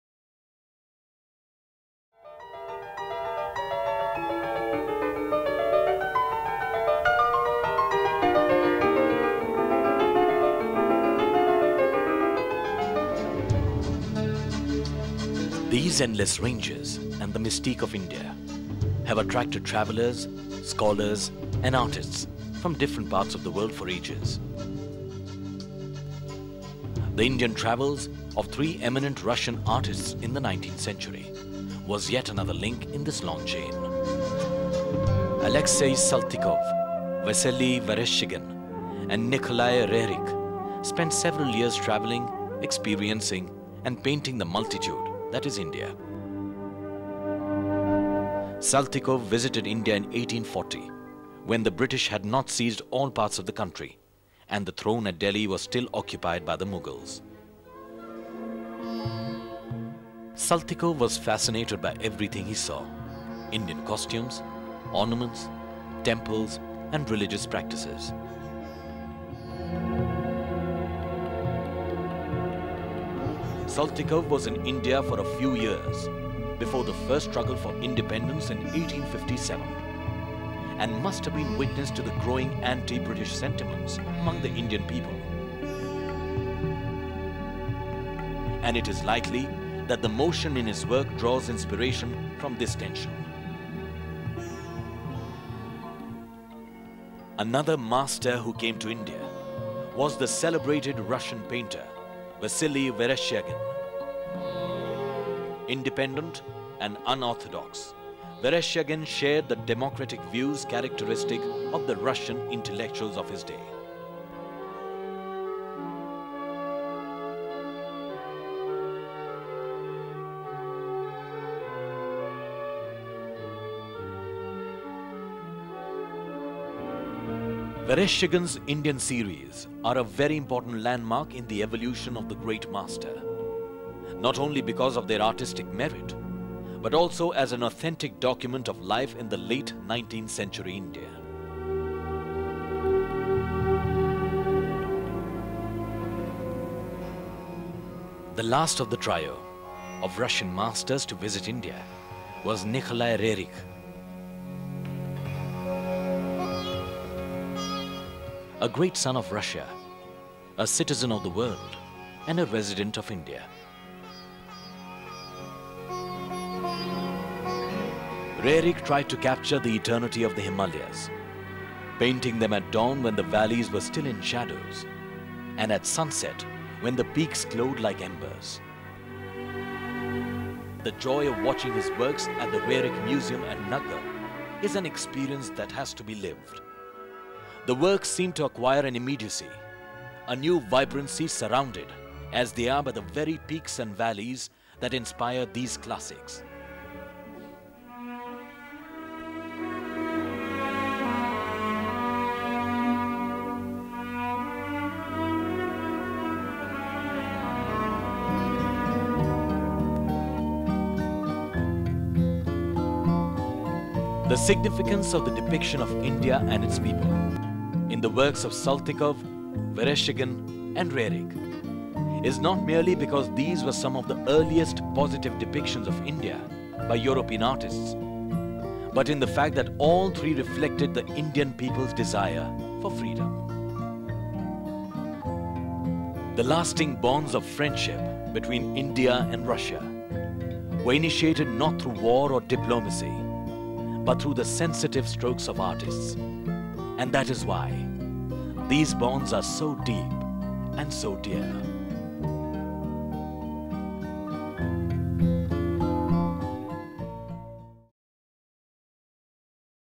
Colours from India (04.19 Mins.)
'Colours From India' is about the three Russian painters who came to love India as their own, which reflected in myriad nature in their works. Russians saw India first through the paintings of Saltykov. He painted everything he saw: common people, Nawabs, processions in Kanchipuram, during his two visits in 1841 and 1845. Vasili Vereshchkegin, inspired by Saltykov's paintings travelled to India. He was more fascinated by the architecture. Nikolai Roerich, unlike Saltykov and Vereshchkegin, lived in India for a longer period, and thus witnessed British imperialism in India. His paintings reflected India's desire for freedom. Through the works of these three artists, 'Colours from India' depicts the bond of friendship between the two nations that was not initiated by war or diplomacy, but by the sensitive strokes of artists.